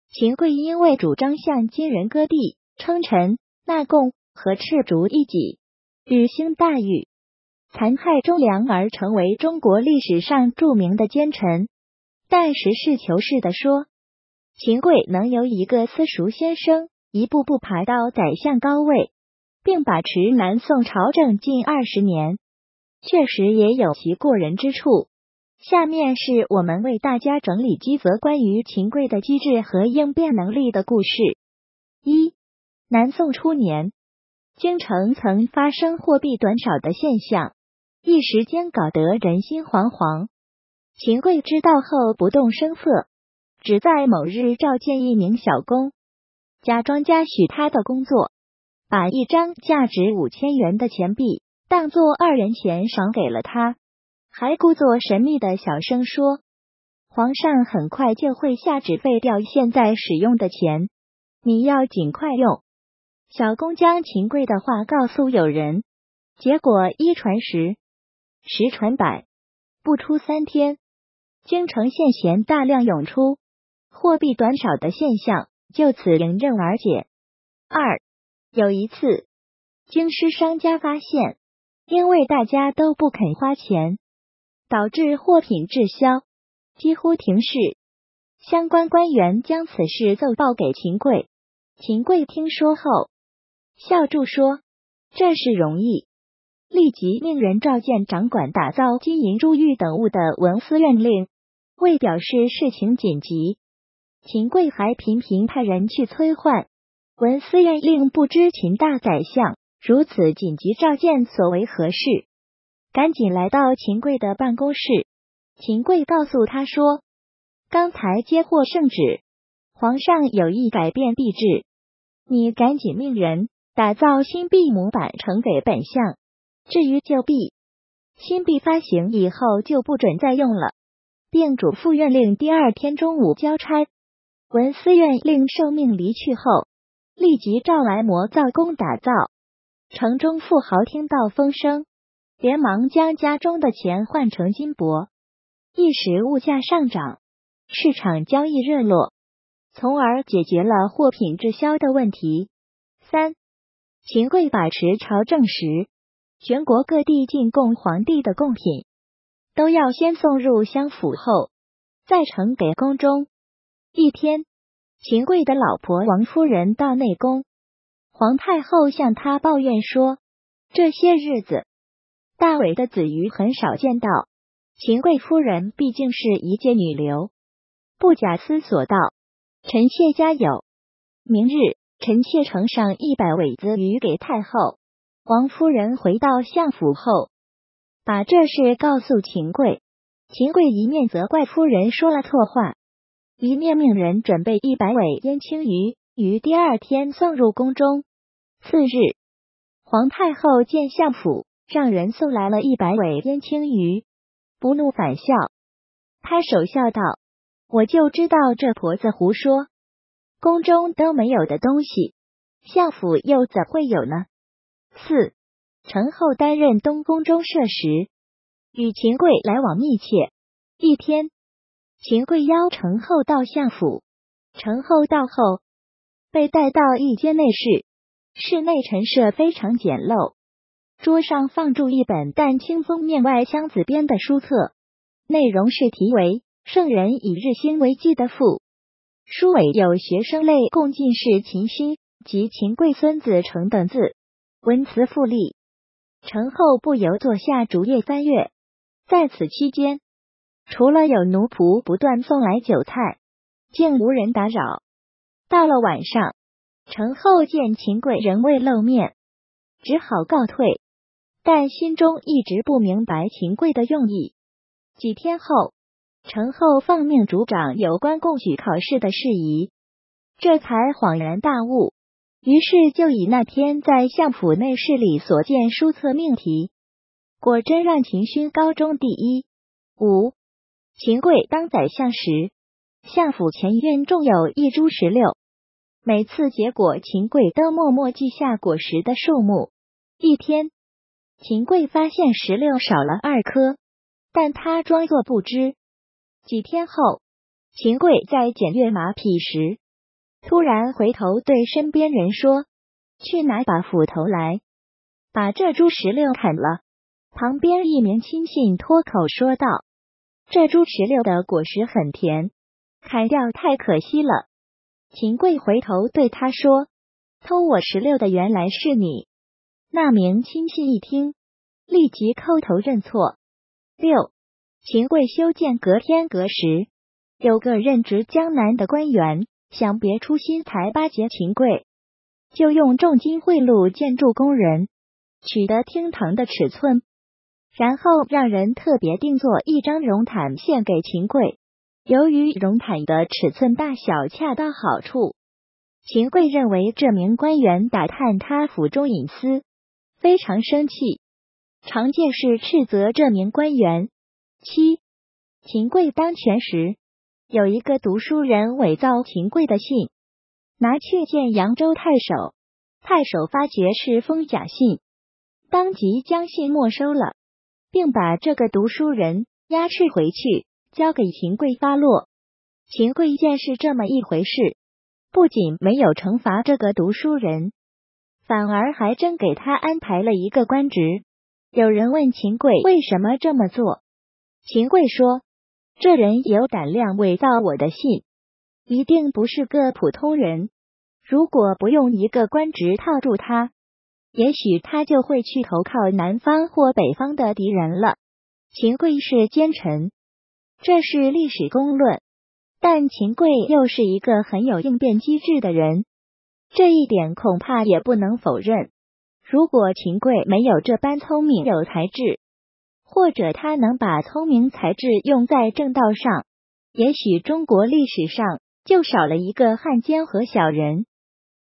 漢奸秦檜的機智和應變能力有多強？這七個歷史小故事告訴你
但實事求是地說，秦檜能由一個私塾先生一步步爬到宰相高位，並把持南宋朝政近二十年，確實也有其過人之處。